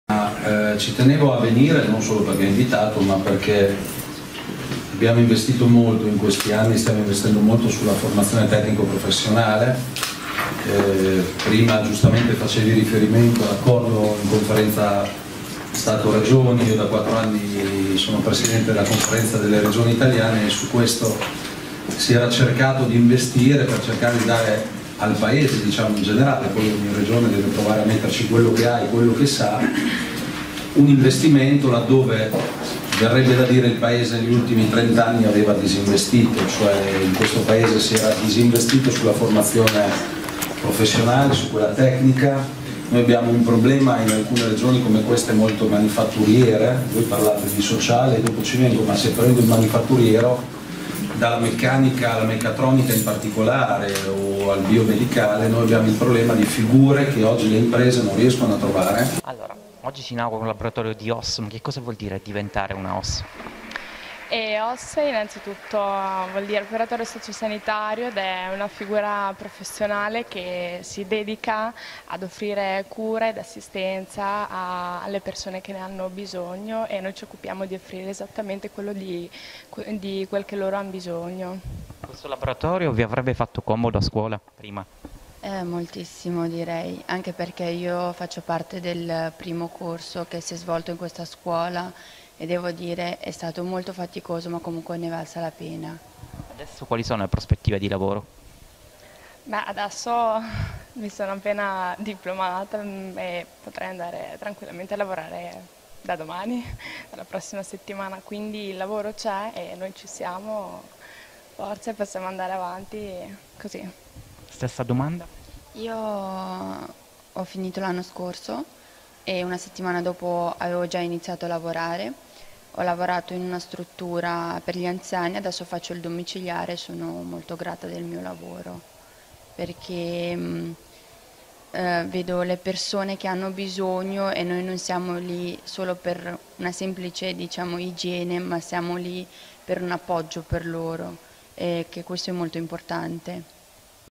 Modena, al Cattaneo un'aula per simulare il servizio Oss in ospedale
Un nuovo laboratorio dotato di tutte le più importanti attrezzature, per mettere in pratica le conoscenze teoriche e prepararsi alla professione di Operatore Socio Sanitario. L'Istituto Cattaneo-Deledda - il primo nella provincia di Modena ad aver attivato il percorso OSS, nato da un protocollo di intesa tra l'Ufficio scolastico regionale e la Regione Emilia-Romagna - ha inaugurato sabato 7 dicembre uno spazio attrezzato per questo indirizzo di studi, realizzato all'interno della sede principale della scuola e perfezionato grazie al progetto europeo Erasmus+KA1, a cui partecipano i docenti dell'istituto. Video Benito Benevento